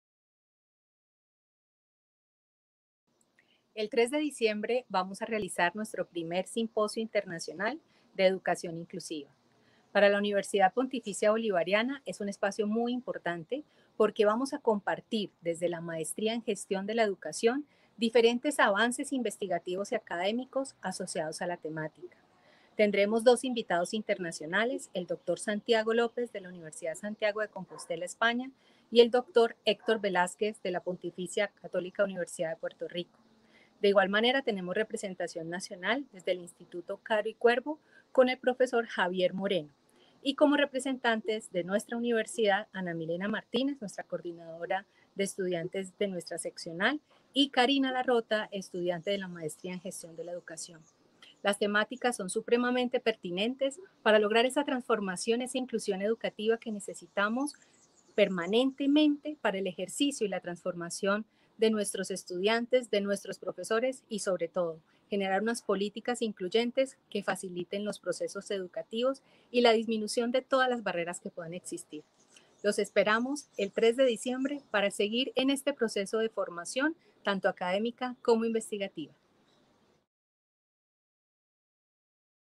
Invitación 1er. Simposio Internacional en Educación Inclusiva
La Dra. Ana Fernanda Uribe Rodríguez, Vicerrectora Académica UPB,  invita al 1er. Simposio Internacional en Educación Inclusiva.

El próximo 3 de diciembre se llevará a cabo el Primer Simposio en Educación Inclusiva S.I.E.I. organizado por la Universidad Pontificia Bolivariana, Seccional Bucaramanga.

El evento tiene como objetivo principal “Fortalecer los procesos de educación inclusiva en los distintos actores que intervienen en el proceso formativo de los estudiantes en cualquier nivel académico” y será desarrollado en el marco del día internacional de la Discapacidad, delimitándola a temas de educación.